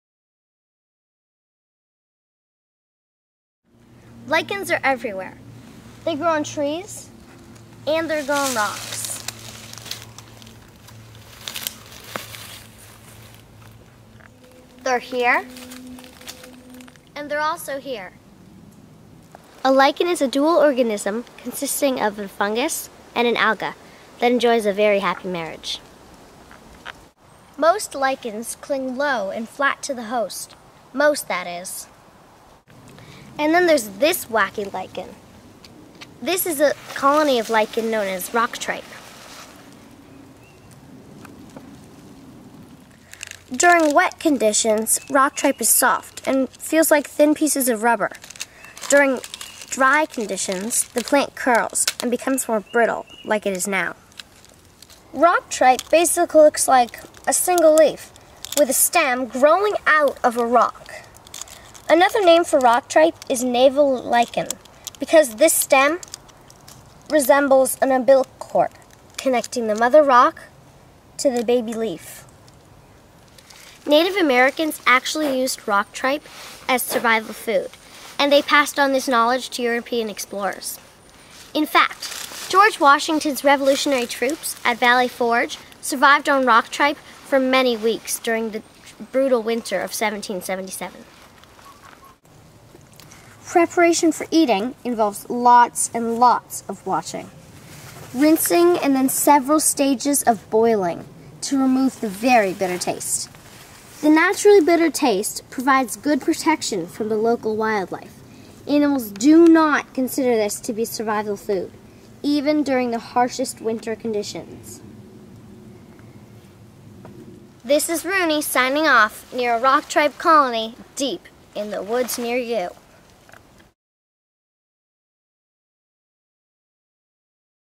Rock Tripe "In the Woods Near You"
Roonie looks for lichens and finds a rock tripe colony